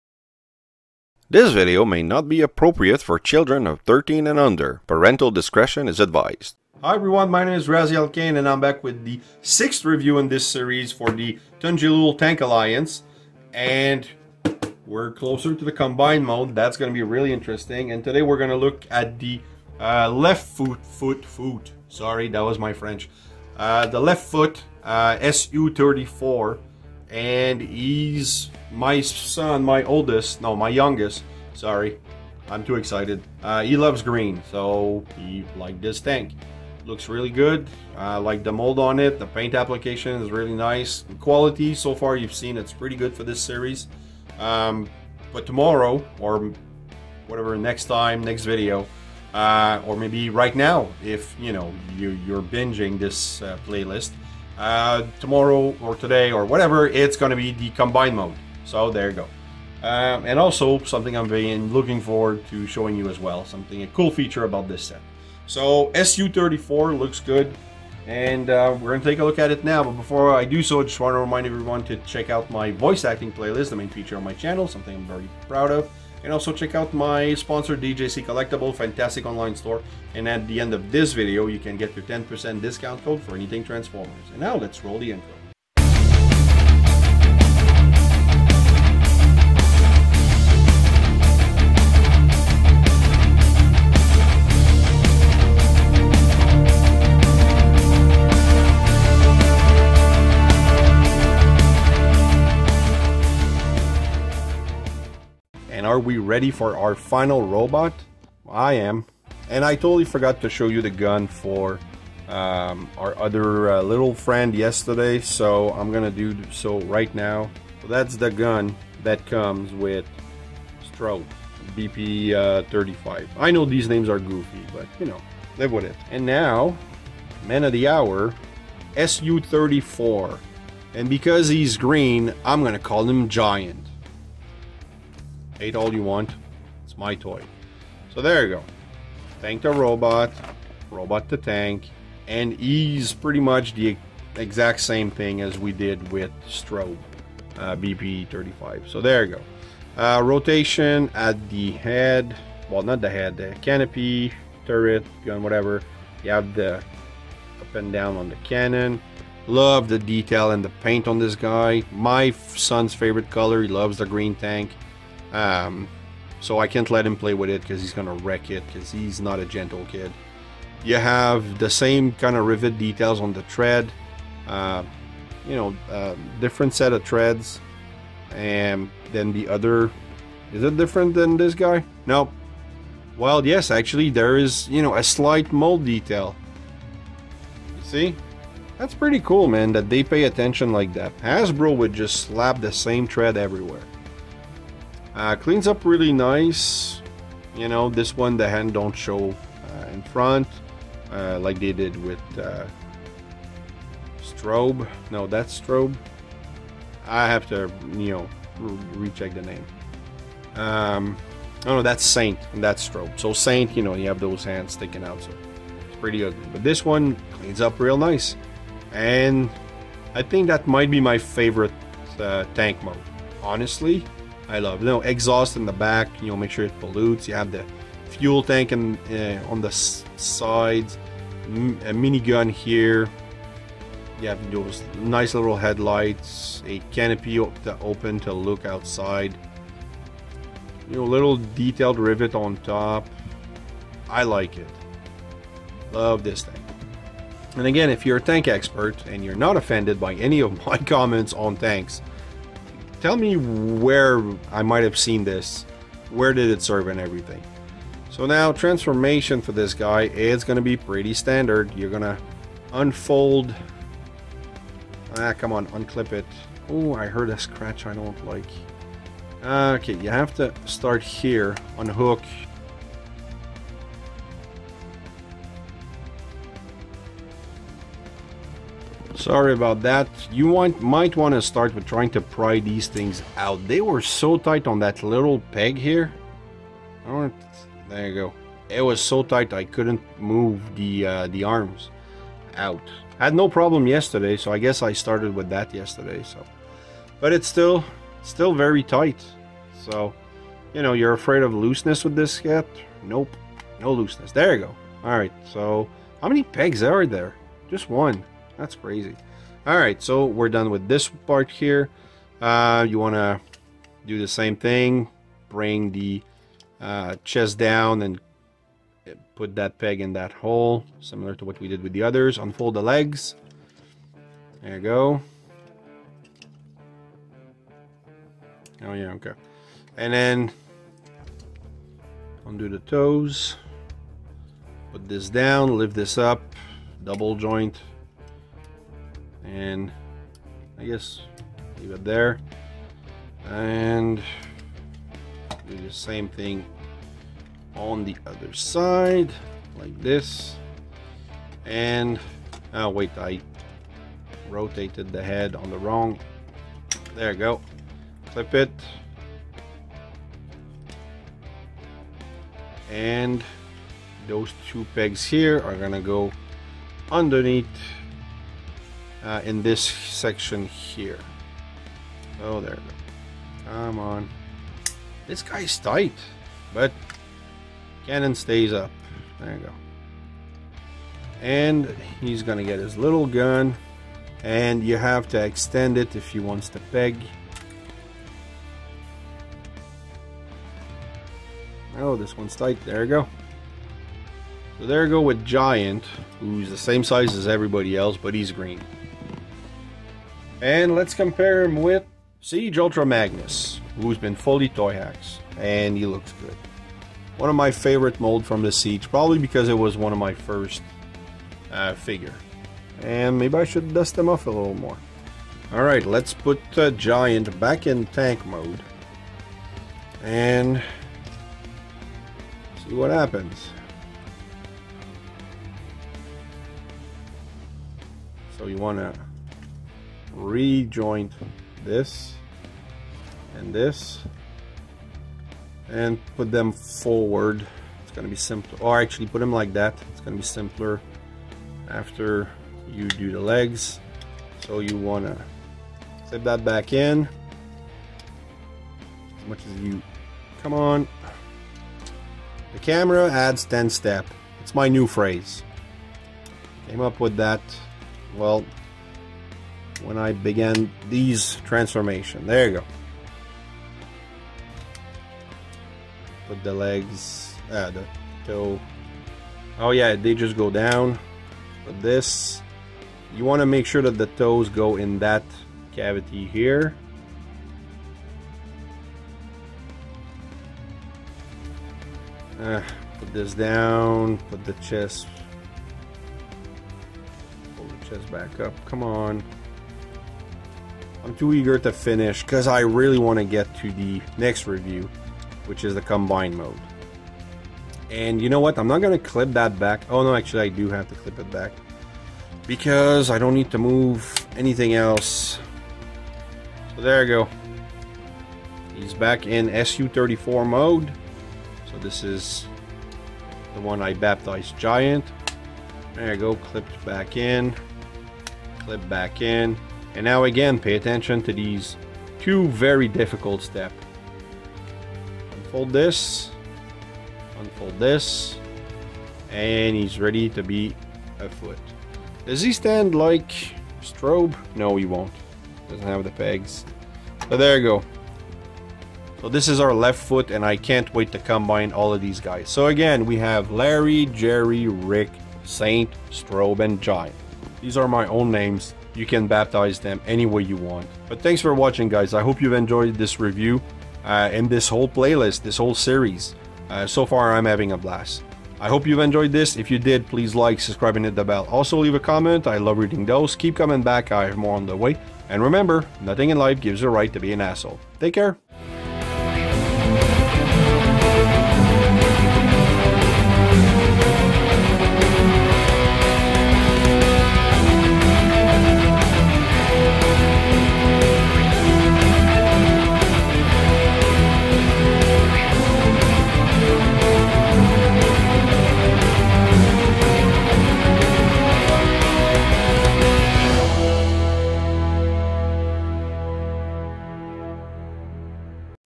Toy Review - Tunjilool Tank Alliance SU 34 (Complete Transformation back and forth)
A birthday gift from my oldest, and I'm in love with this set. Come take a look at this series of review of the Tunjilool Tank Alliance!

for all your transformers and Collectibles needs!

Into and Outro
Intro, watermark and Icon Photo by Jorge Rojas on Unsplash
We Like you too Photo by Yoav Hornung on Unsplash